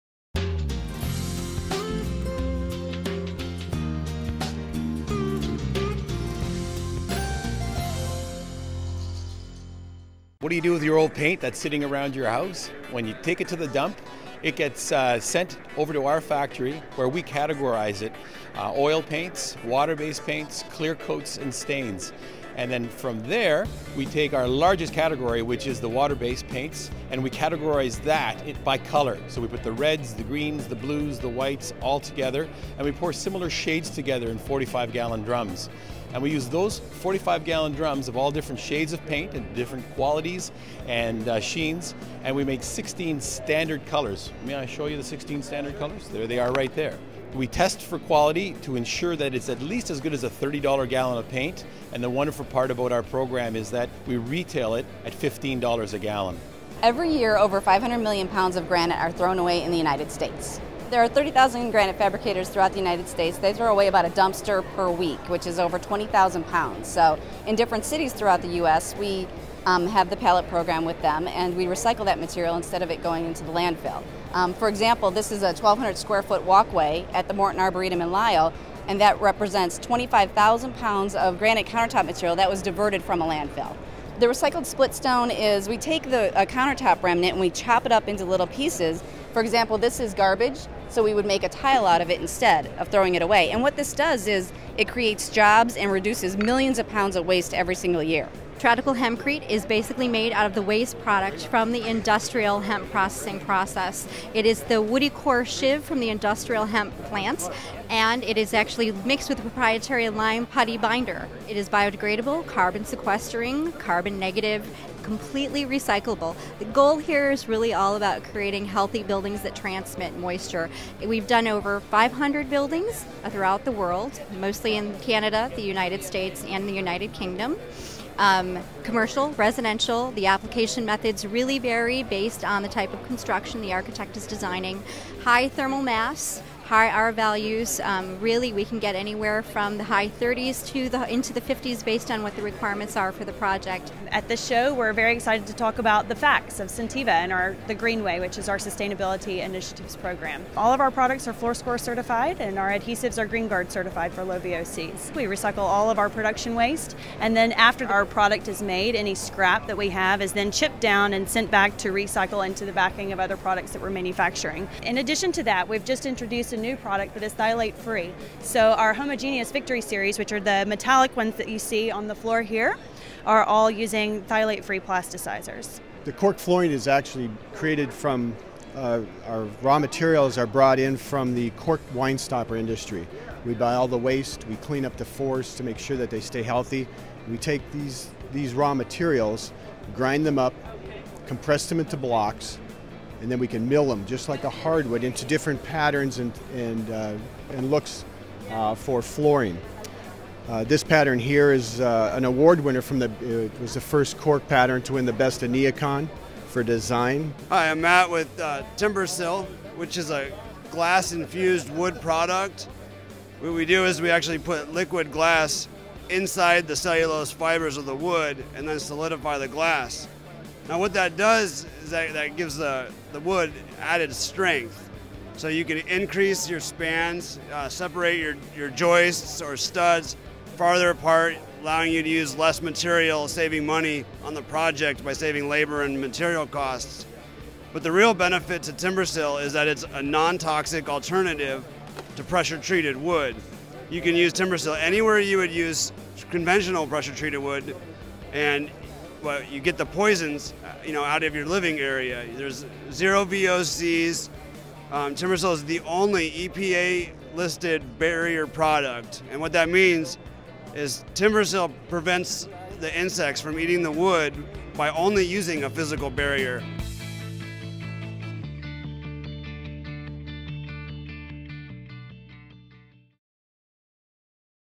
The Greenest Materials of Green Build 2011
The Greenest Materials of Green Build 2011.  Learn the hottest trends and tech in green building. Join Green Building Pro as we take you Toronto for Green Build the largest Green Building expo in the world. Watch all 3 videos and stay up to date with the newest breakthroughs in Green Building for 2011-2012. Video 2 show cases innovations in green and recycled building materials.